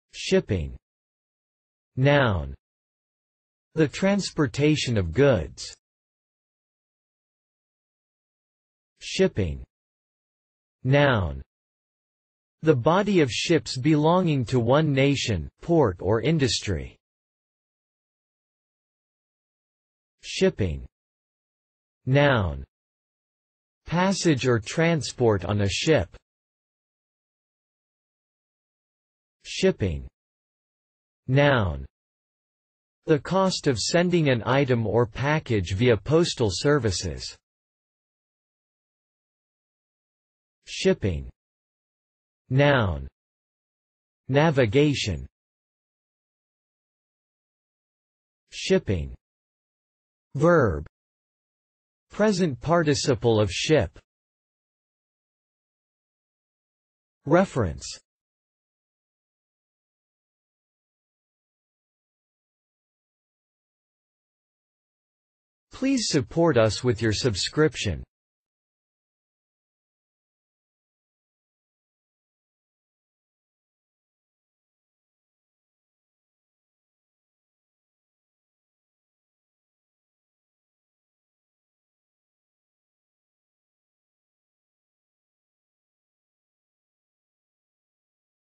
Shipping | Meaning of shipping
See here, the meanings of the word shipping, as video and text.

shipping (noun)
    The transportation of goods.

shipping (noun)
    The body of ships belonging to one nation, port or industry.

shipping (noun)
    Passage or transport on a ship.

shipping (noun)
    The cost of sending an item or package via postal services.

shipping (noun)
    Navigation.

shipping (verb)
    Present participle of ship.

Reference:
    shipping